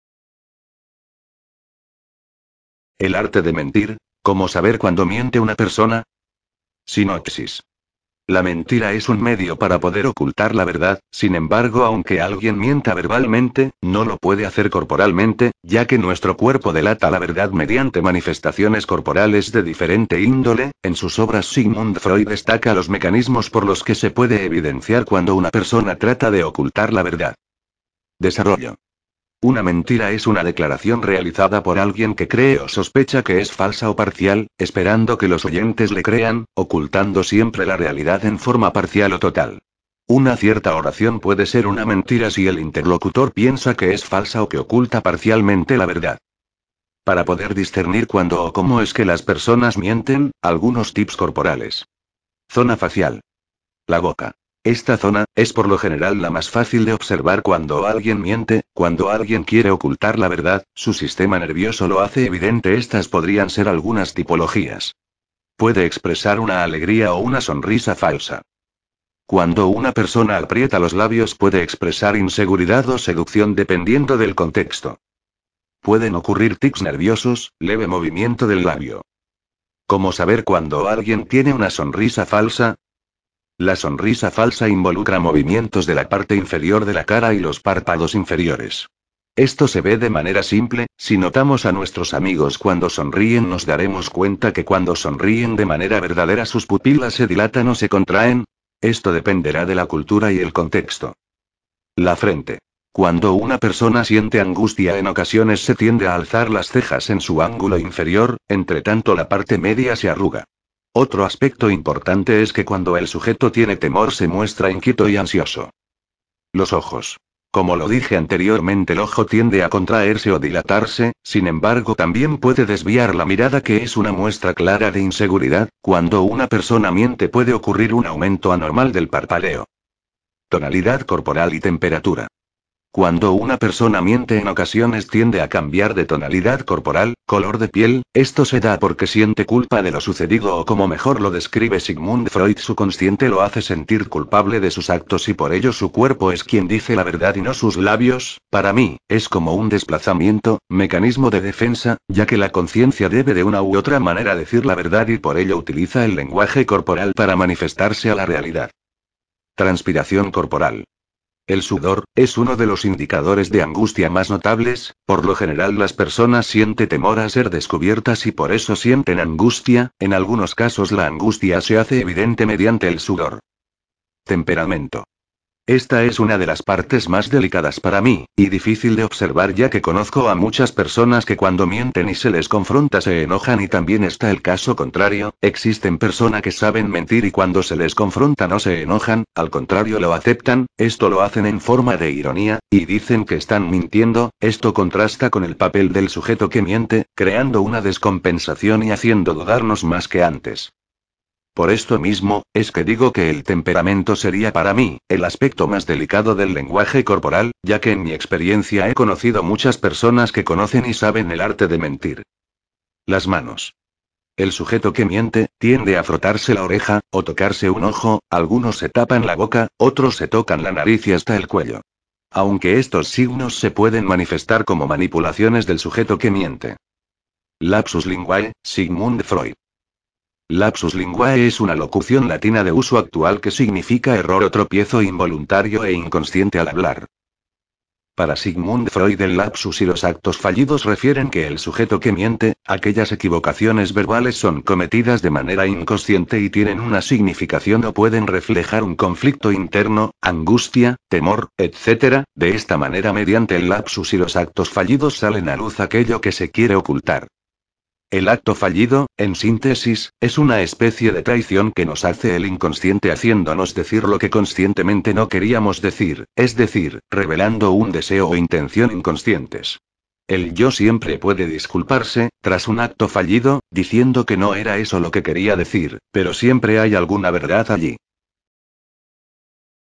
El Arte de Mentir - ¿Cómo saber cuándo miente una persona?
Detectar cuando alguien está mintiendo puede ser un desafío, ya que no existe una señal universal que indique con certeza que alguien está mintiendo. Sin embargo, existen ciertos indicadores de posible engaño que puedes observar.